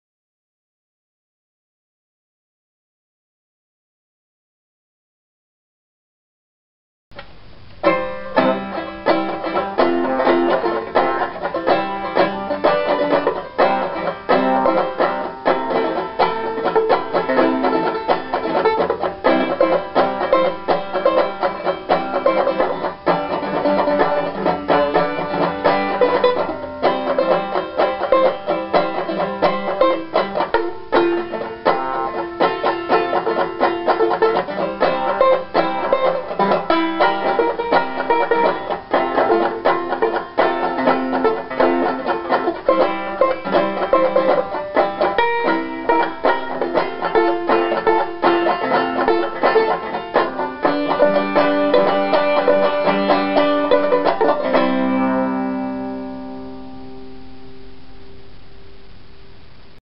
Vintage 1931 Weymann Style 50 Tenor Banjo
This is a demonstration video of a Vintage 1931 Weymann Style 50 Tenor Banjo that we currently have on eBay.  You can view close-up pictures, read the description, and check out more items at our ebay